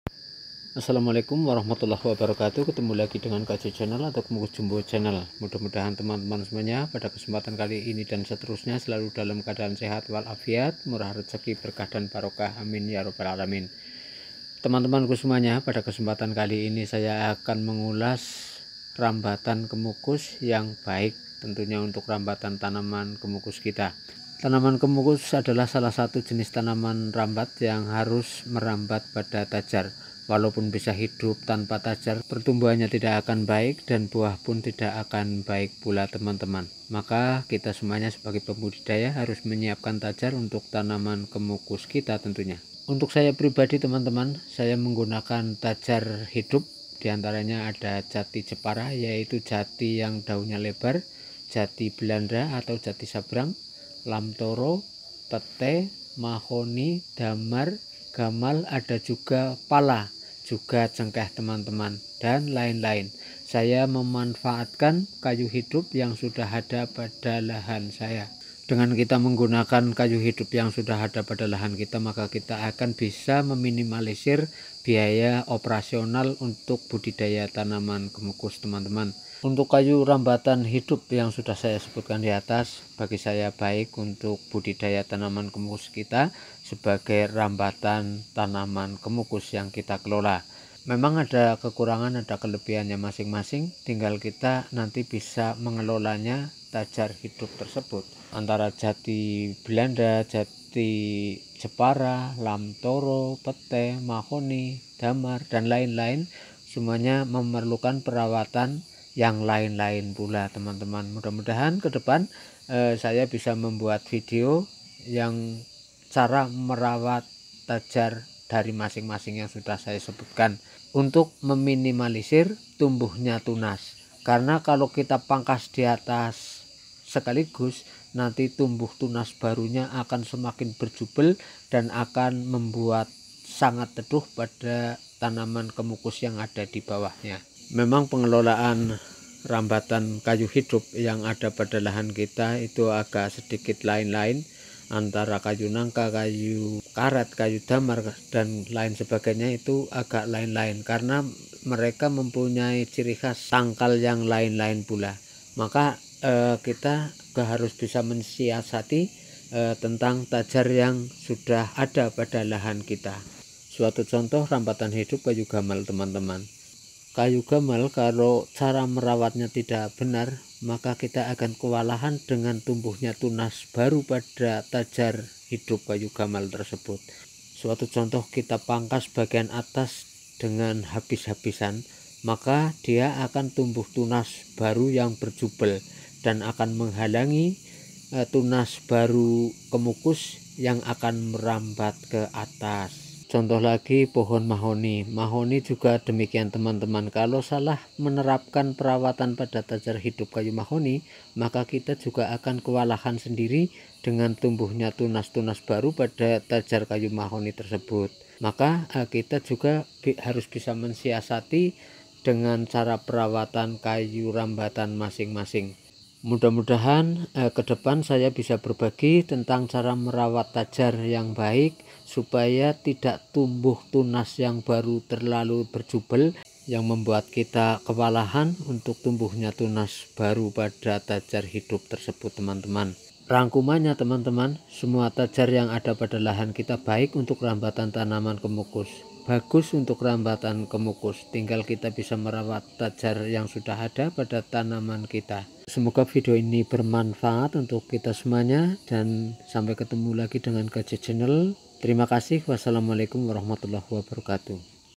Rambatan (tajar) kemukus yang baik//Rambatan kayu hidup.
Rambata kemukus sangat berpengaruh pada pertumbuhan dan produktifitas kemukus. Semua kayu hidup bagus untuk rambatan kemukus. Tetapi perlu penanganan tepat untuk mempermudah perawatan berikutnya. Rambatan kemukus memakai kayu hidup. Bisa untuk memangkas biaya oprasiona dalam budidaya tanaman kemukus.